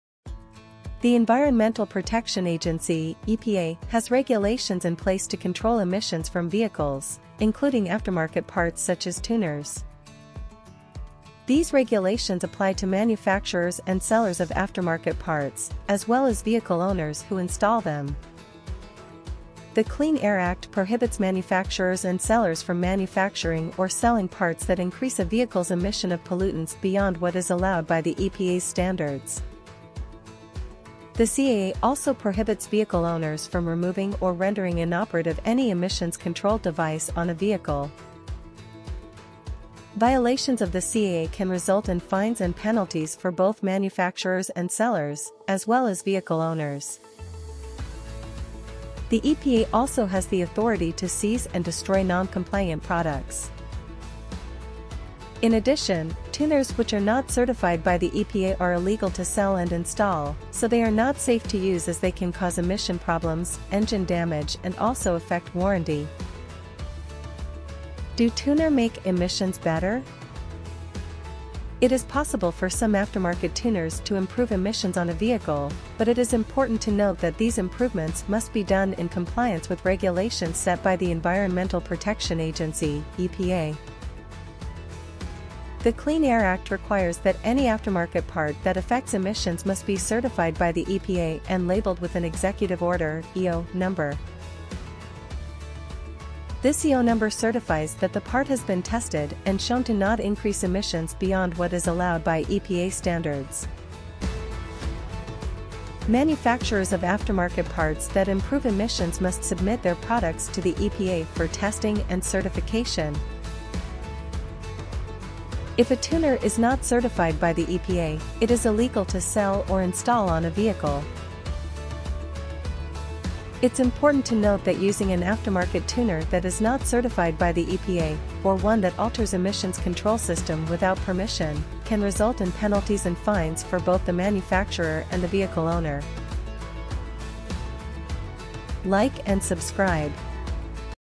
car tuner
I ask A,I. about car tuners